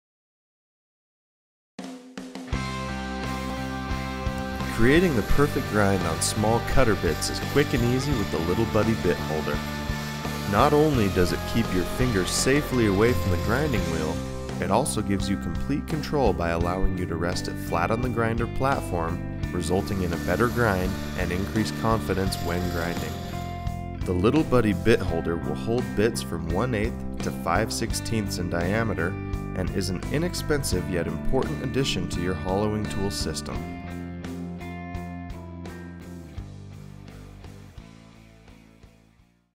Little Buddy Bit Holder (Sharpening Bit Holder)
Sharpen small bits safely with the Little Buddy Bit Holder.

This simple little sharpening jig keeps your fingers safely away from the grinding wheel while you sharpen small cutter bits. The double-ended design allows you to hold square cutter bits from 1/8" through 5/16". Measures 1 3/4" x 1" x 5/8". Allen wrench and instructions included.

- SOCIAL MEDIA -